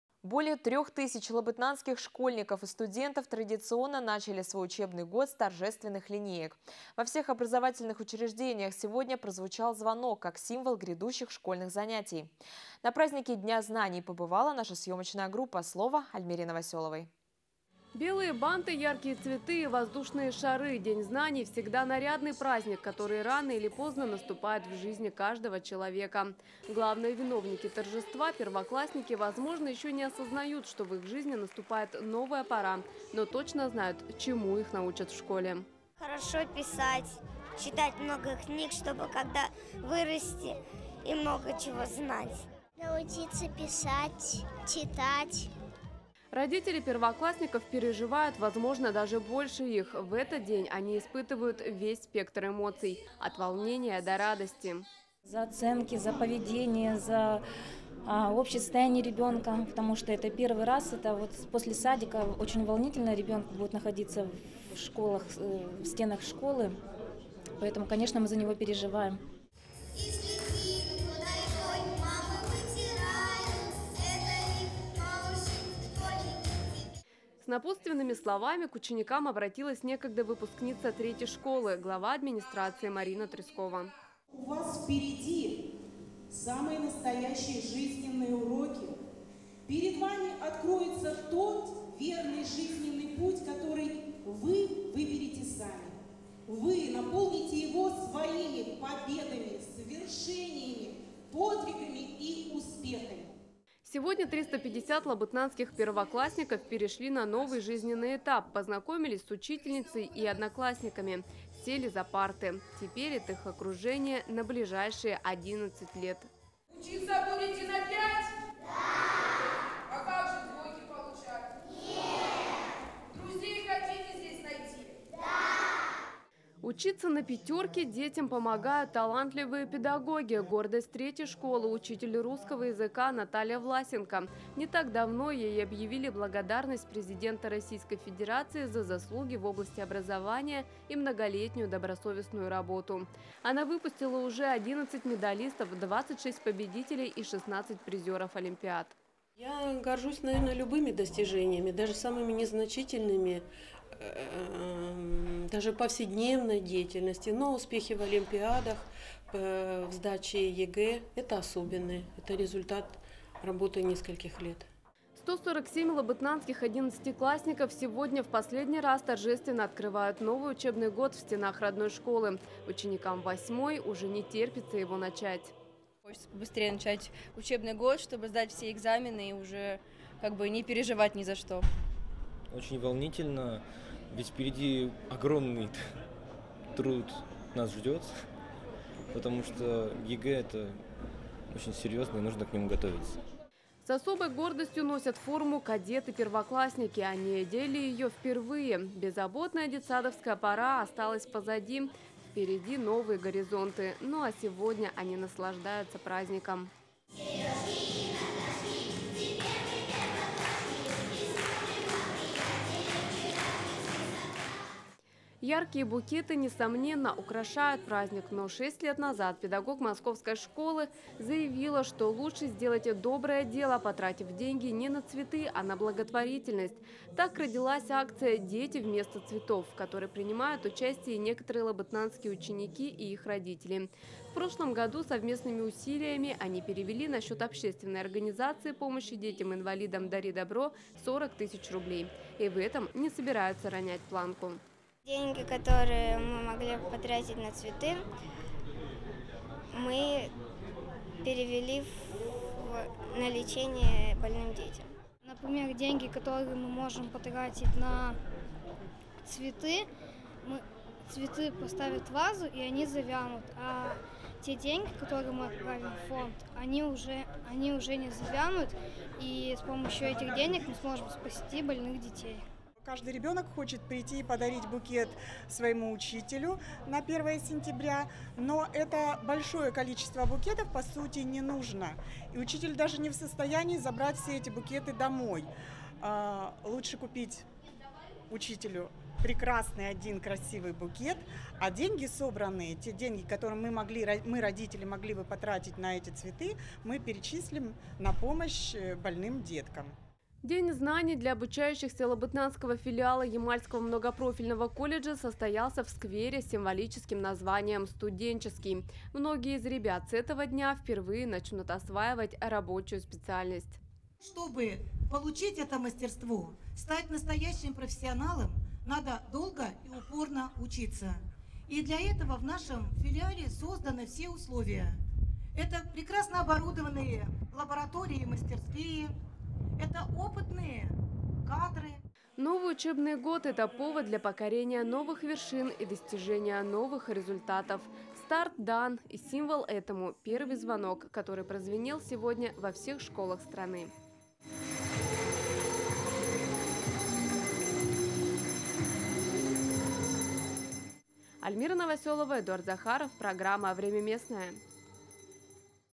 День знаний
Более трёх тысяч лабытнангских школьников и студентов традиционно начали свой учебный год с торжественных линеек. Во всех образовательных учреждениях сегодня прозвучал звонок, как символ грядущих школьных занятий.